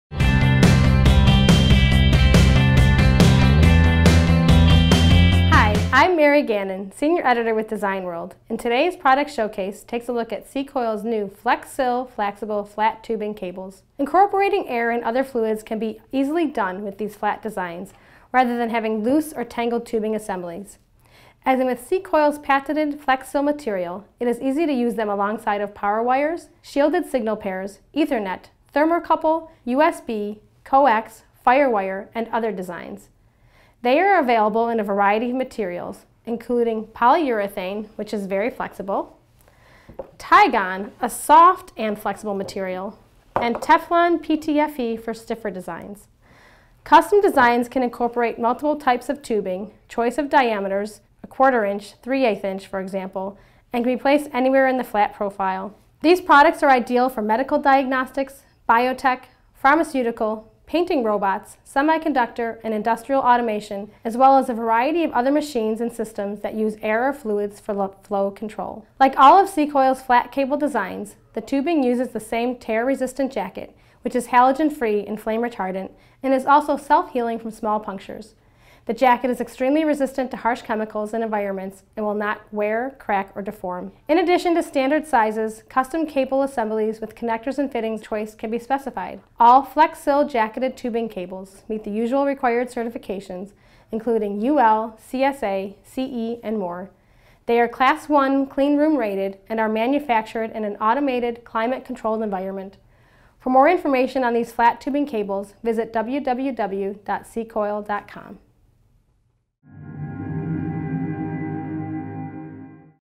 Product Spotlight - Cicoil Flexx-Sil Cables
Design World Senior Editor Mary Gannon discusses Cicoil's Flexx-Sil flexible, flat tubing cables.